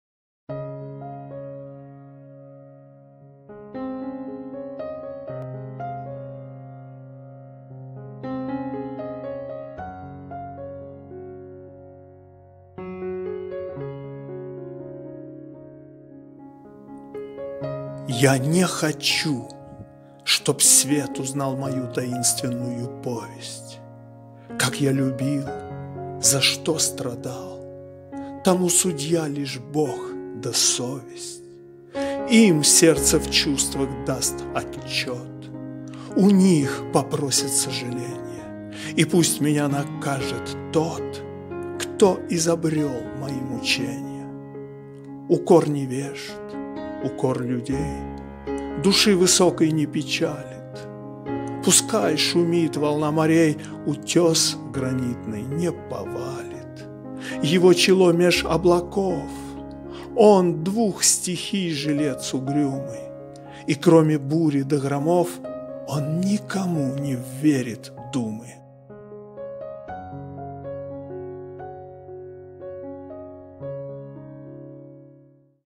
Михаил Лермонтов - Я не хочу, чтоб свет узнал:Стих
Стихи:Михаил Лермонтов;
Читает:Виктор Санин;